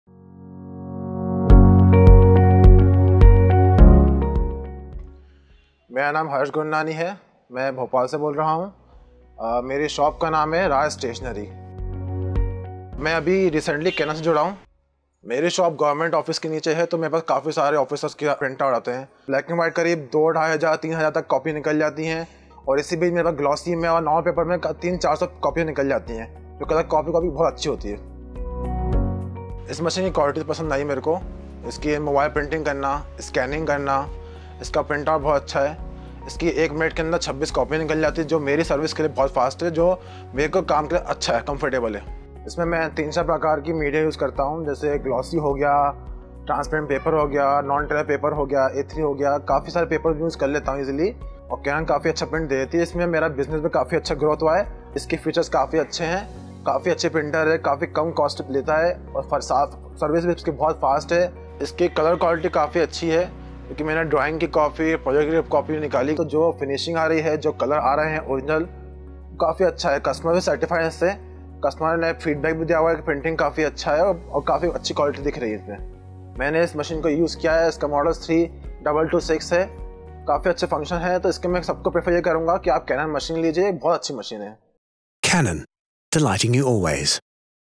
iR C3226 Testimonial : Raj Stationary, Bhopal
With an aim to bring Colour to every business across India, we are delighted to share testimonials of copy shop owners who have recently purchased the new Canon A3 colour multi-function device iR C3226!